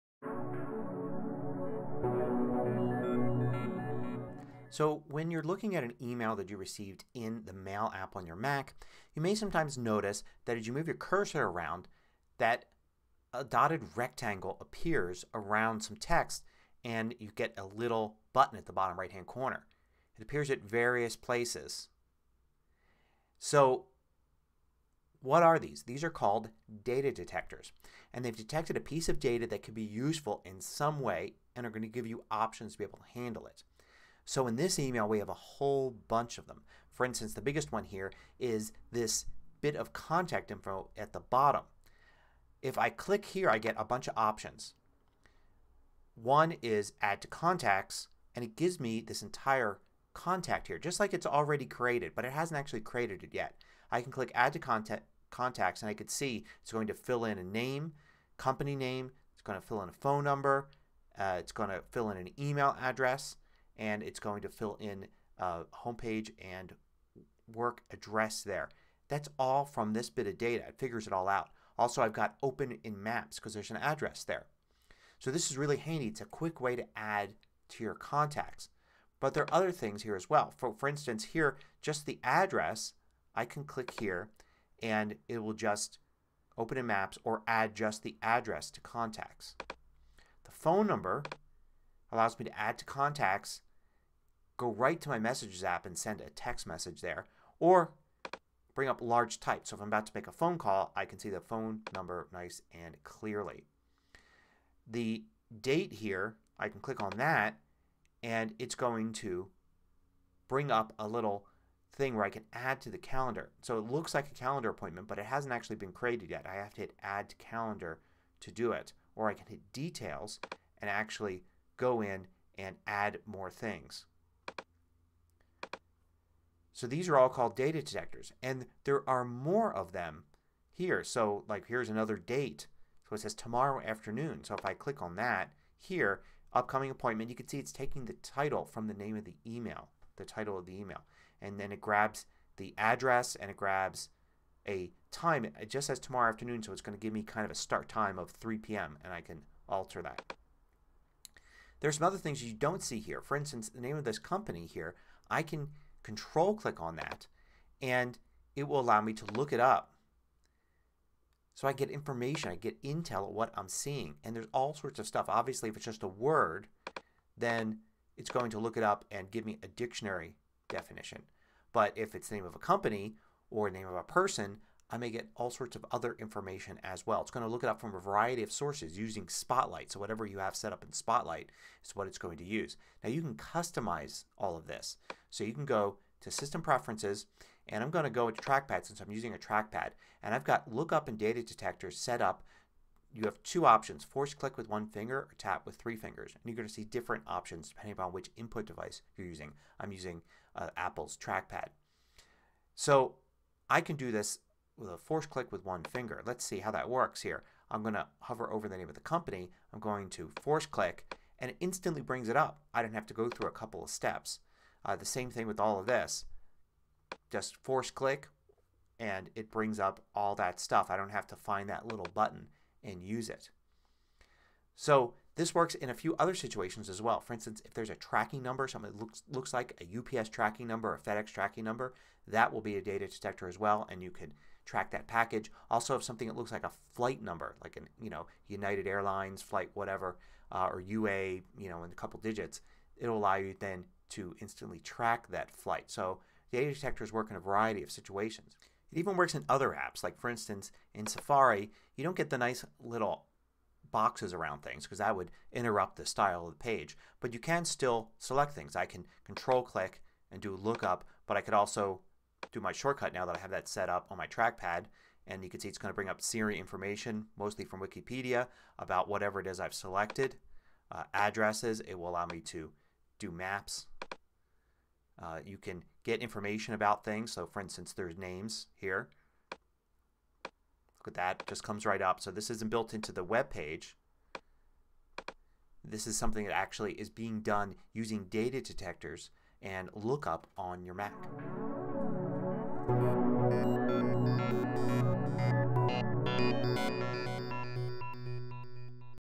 Data Detectors and Look Up (MacMost #1845)
You can create contacts, set up calendar events, and get intel on pieces of text in incoming email, web pages and other documents using Data Detectors and Look Up. This feature of macOS will figure out what information represents, such as a location or appointment time, or business name or person, and allow you to act on it.